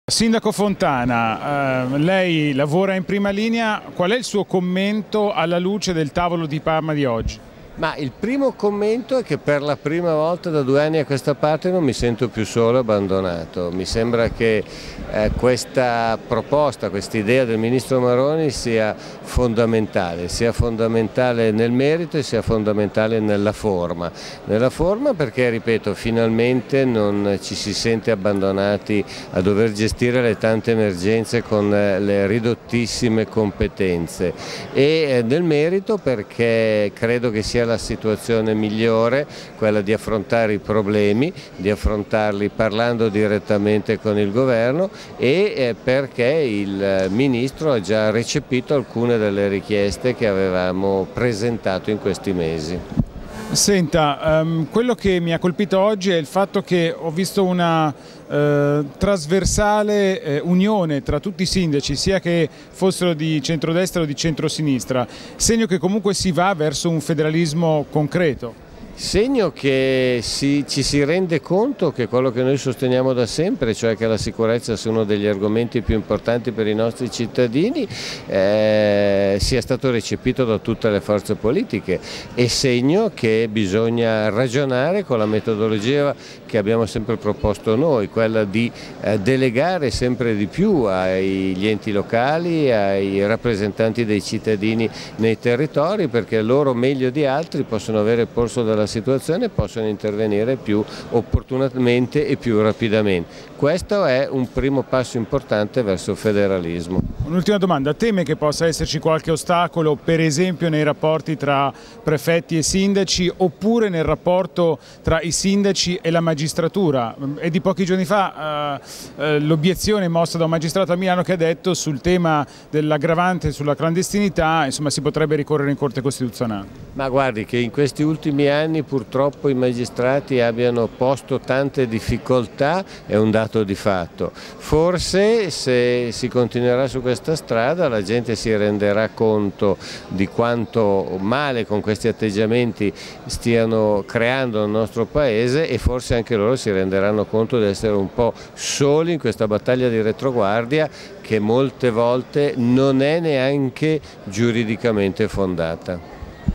Intervista A. Fontana
Commento di Attilio Fontana a margine del l'incontro con il Ministro Maroni. Impressioni e pareri positivi in tema di sicurezza con il nuovo Governo.